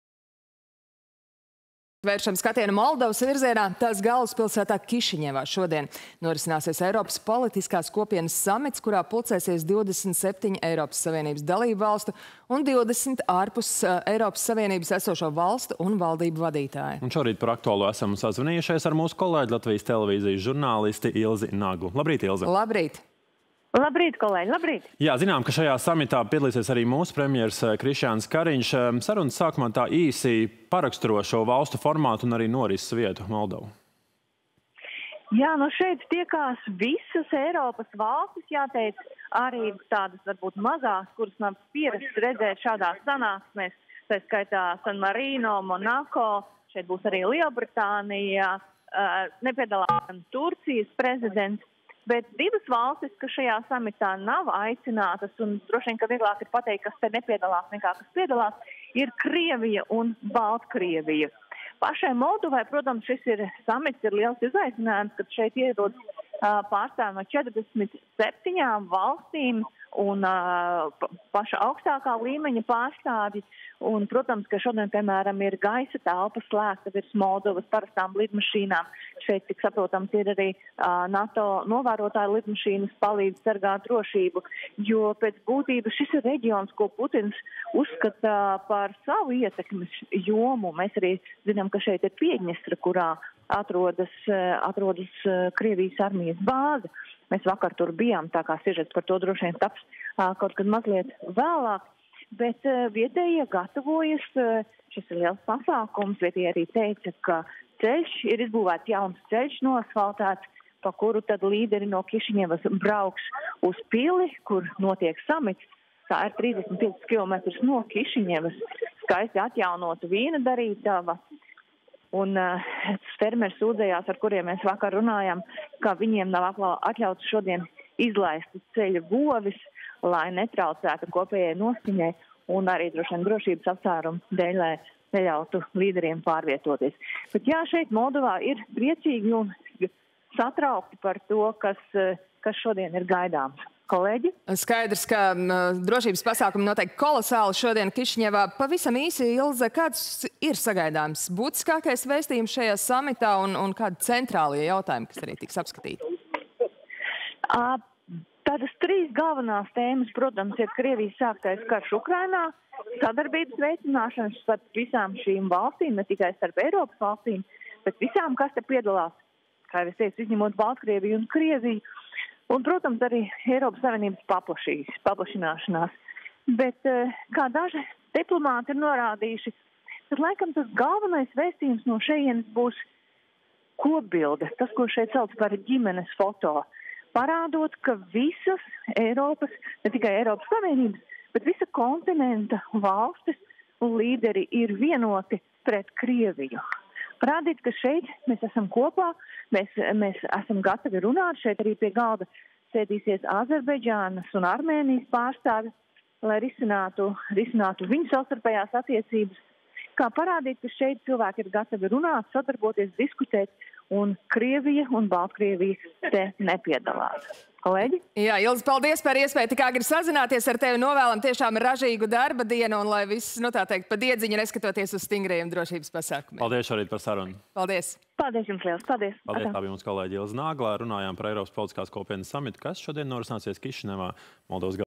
Telefonintervija ar LTV žurnālisti Ilzi Naglu
Šodien Moldovas galvaspilsētā Kišiņevā norisināsies Eiropas politiskās kopienas samits, kurā pulcēsies 27 Eiropas Savienības dalībvalstu un 20 ārpus Eiropas Savienības esošo valstu un valdību vadītāji.
     Un šorīt par aktuālo esam sazvanījušies ar mūsu kolēģi, Latvijas Televīzijas žurnālisti Ilzi Naglu.